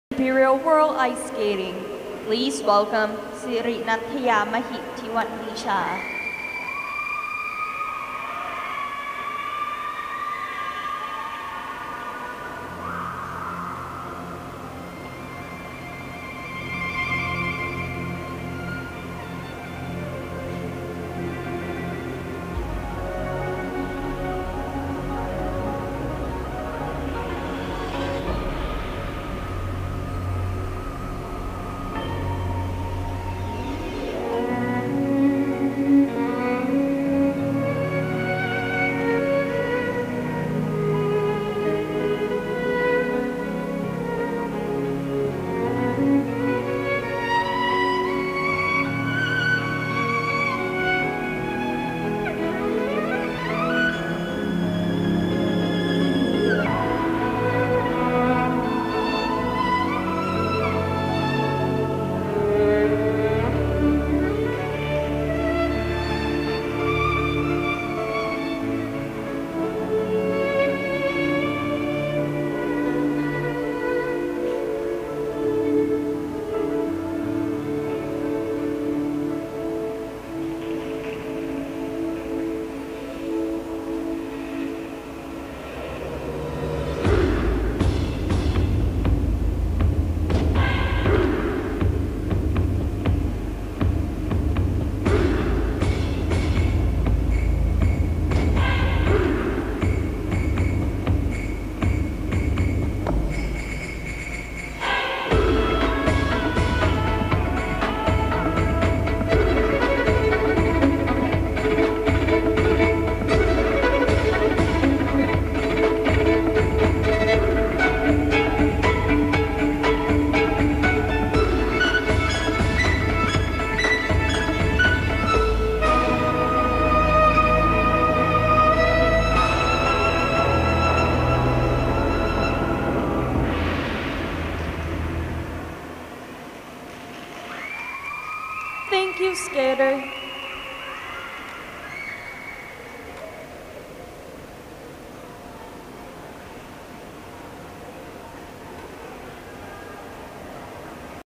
ANI Open Freestyle Bronze 8 years Skate Asia 2019 ISI Figure Skate Competition Bangkok
Skate Asia 2019 ISI Figure skate competition, Bangkok, Thailand
Open Freestyle Bronze, 8 years old
2nd place